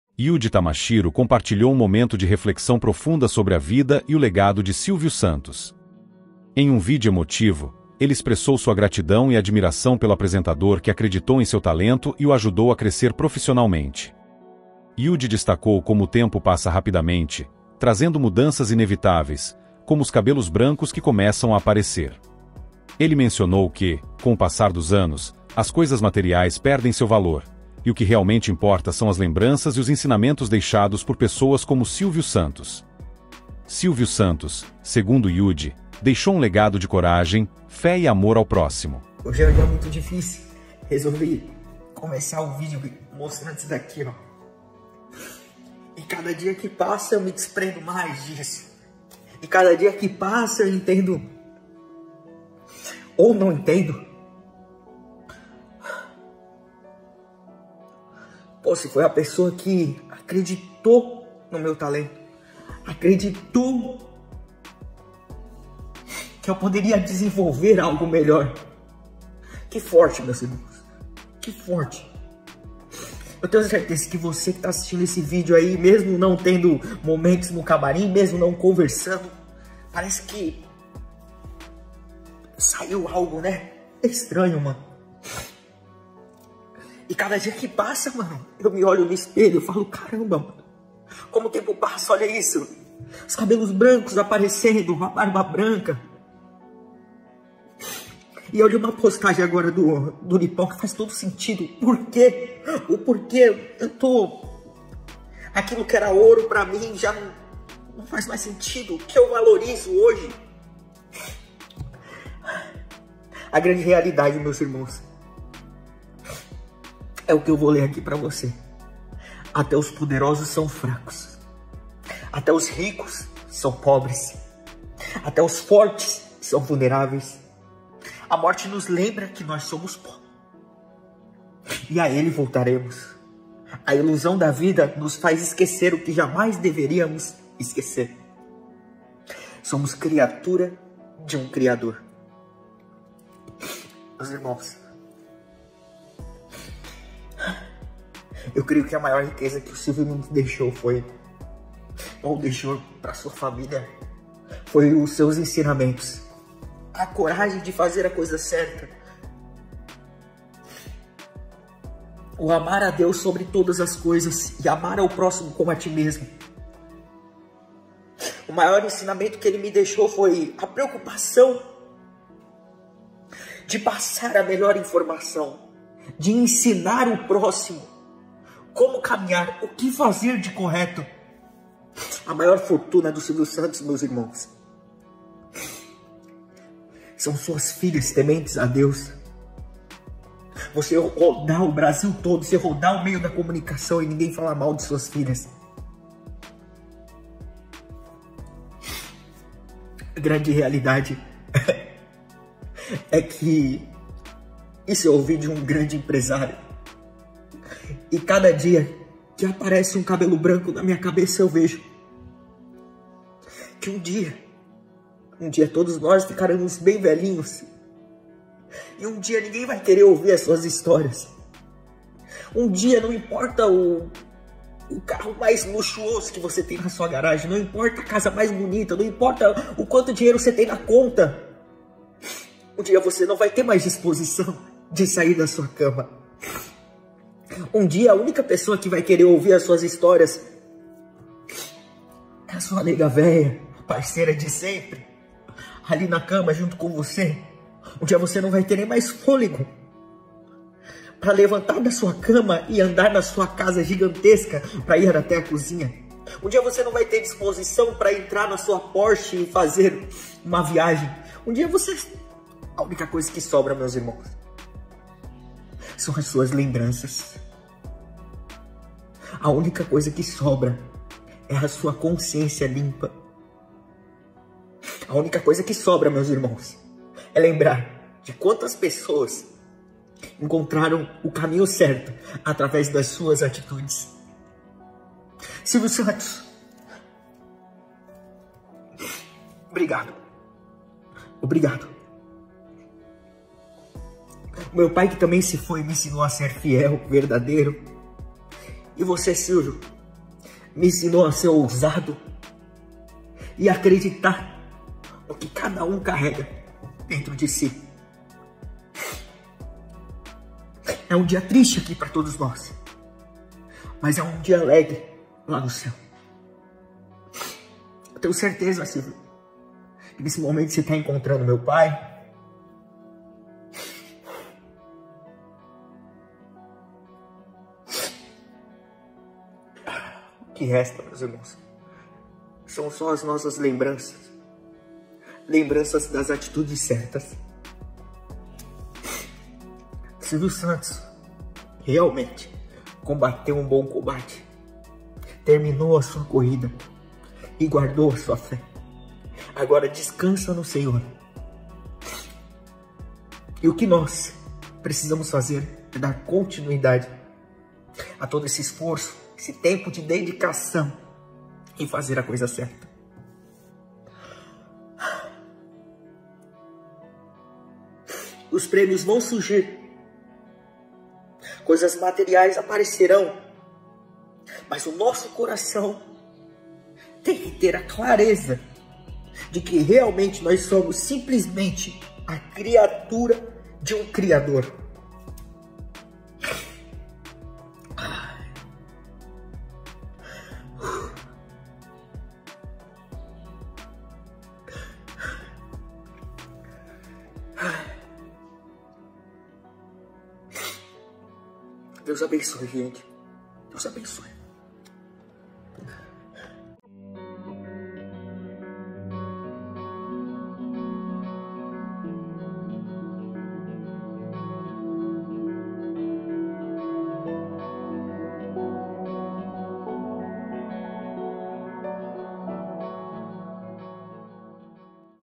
Yudi Tamashiro vai as LÁGRIMAS ao falar de Silvio Santos
Yudi Tamashiro compartilhou um momento de reflexão profunda sobre a vida e o legado de Silvio Santos. Em um vídeo emotivo, ele expressou sua gratidão e admiração pelo apresentador que acreditou em seu talento e o ajudou a crescer profissionalmente. Yudi destacou como o tempo passa rapidamente, trazendo mudanças inevitáveis, como os cabelos brancos que começam a aparecer. Ele mencionou que, com o passar dos anos, as coisas materiais perdem seu valor, e o que realmente importa são as lembranças e os ensinamentos deixados por pessoas como Silvio Santos. Silvio Santos, segundo Yudi, deixou um legado de coragem, fé e amor ao próximo.